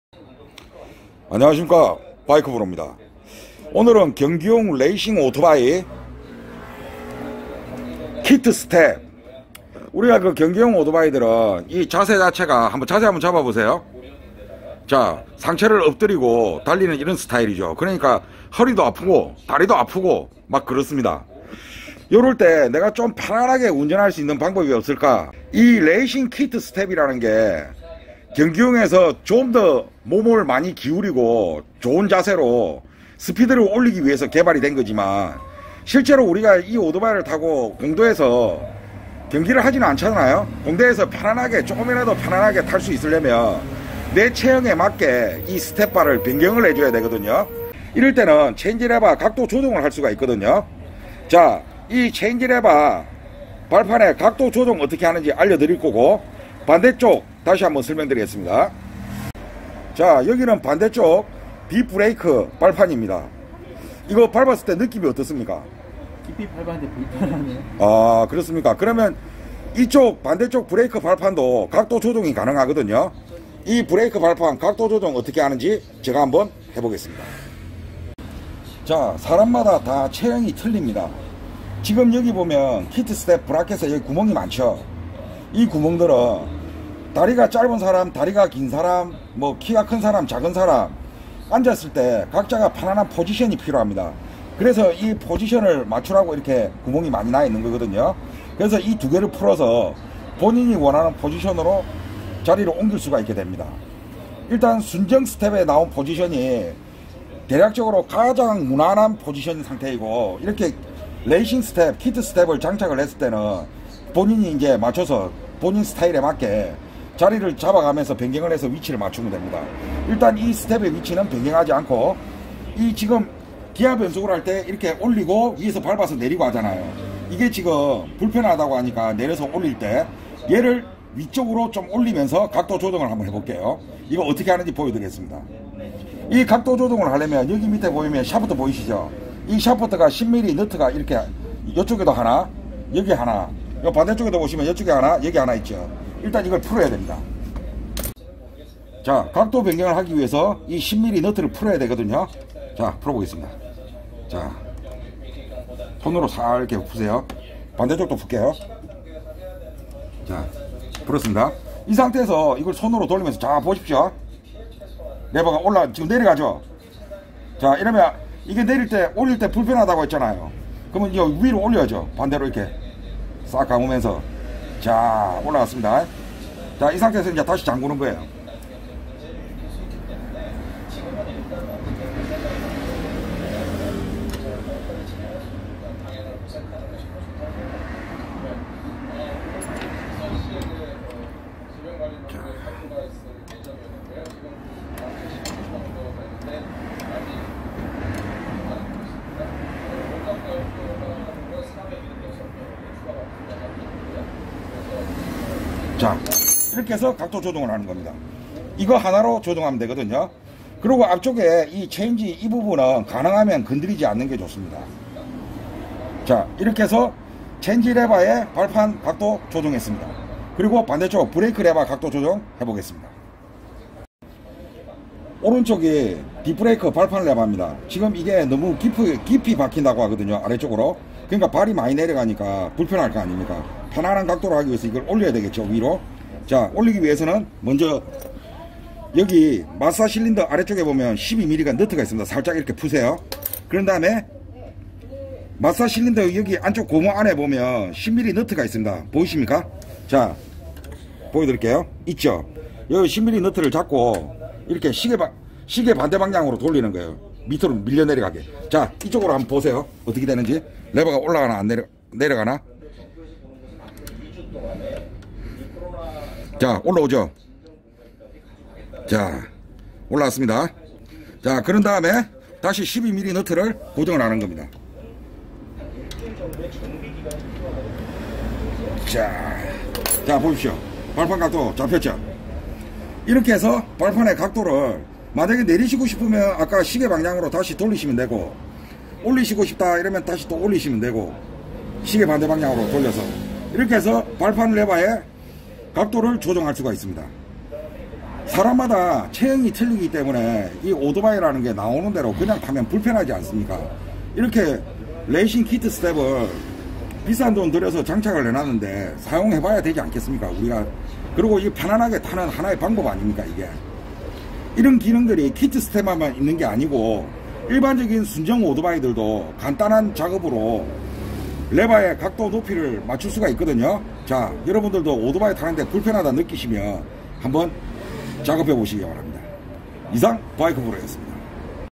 오토바이 레이싱 킷트스텝 기어페달 브레이크페달 각도조정 How To Adjust Your Motorcycle 6R Control Pedal [오토바이 기초팁 15탄]
* 오토바이 발판 스탭 각도조정으로 편한한 세팅하는 방법설명합니다 *
1. 경기용 킷트스탭 주행중 불편한 포지션 발판각도 조정하는방법
2. 각도조정에 필요한 공구설명
3. 킷트스탭은 상하좌우 다양한 위치변경이 가능하다는 설명
4. 좌측 기어변속 체인지페달 위아래로 각도 변경하는 방법설명
    (순정 기어변속 체인지페달도 변경가능)
5. 우측 브레이크 페달 위아래로 각도 변경하는 방법설명
   (순정 브레이크 페달도 각도 변경가능)
6. 여러종류의 백스탭도 같은 방법으로 각도 변경이 가능합니다

- 바이크 : KAWASAKI NINJA6R -